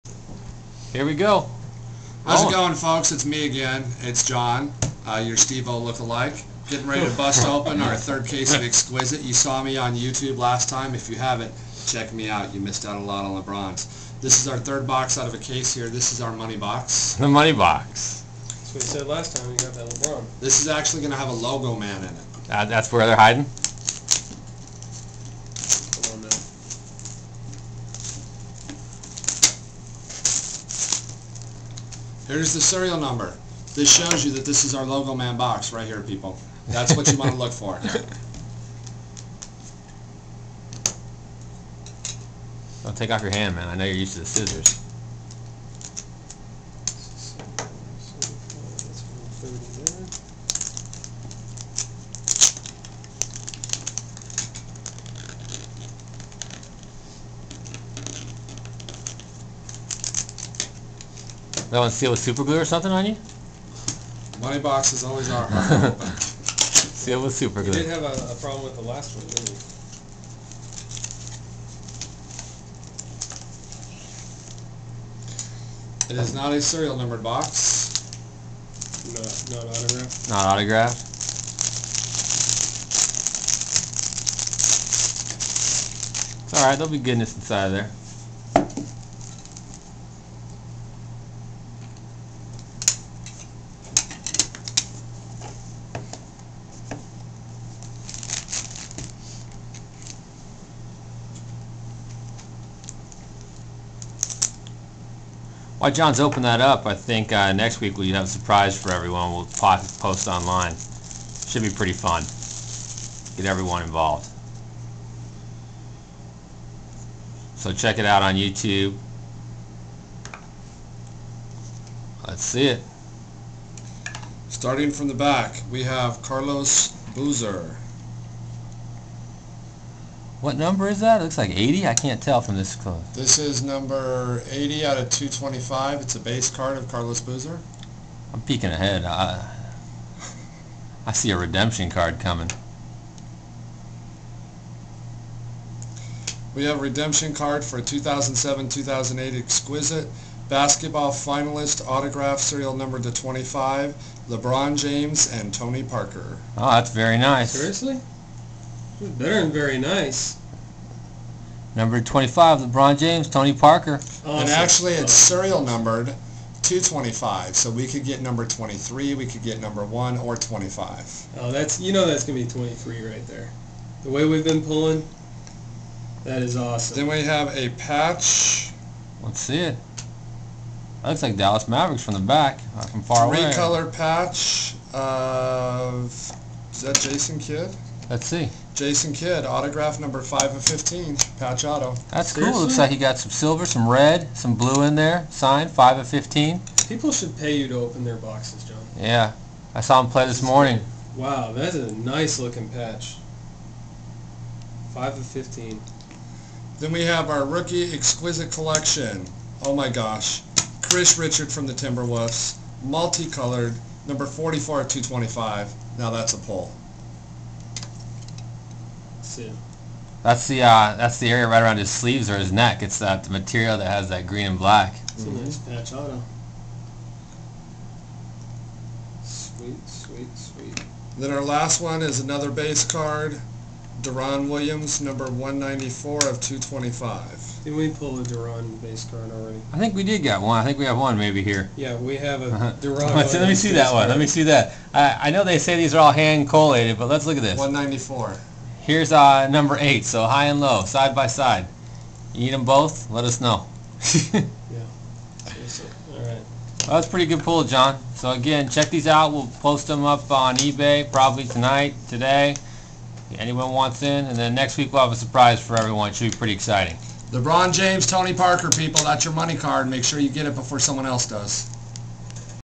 2007-08 Exquisite Basketball CASE Break - Box 3
After the incredible Lebron James 23/23 box, we decided we needed to do an Exquisite case break.  We ended up with some more very nice cards.

Don't forget to check ebay for all the good stuff: